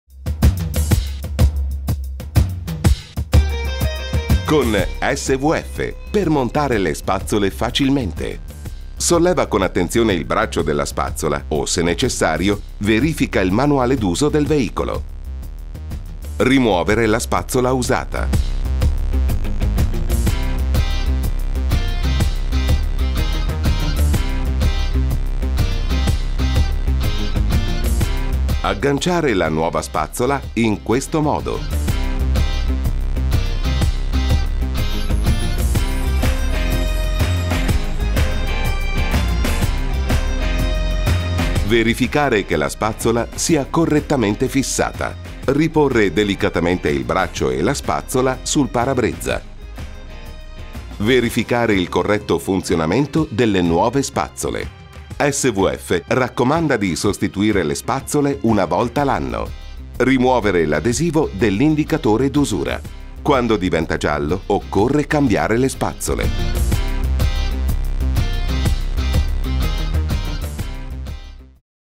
SWF montaggio spazzola con adattatore U
Con SWF, monta facilmente la tua spazzola!
1-Solleva il braccio della spazzola
2-Rimuovi la spazzola montata
3-Aggancia la nuova spazzola.
4-Assicurati il corretto fissaggio della nuova spazzola.
5-Appoggia sul parabrezza il braccio e la nuova spazzola.
6-Assicurati il corretto funzionamento della spazzola.

SWF raccomanda di sostituire le spazzole una volta all'anno!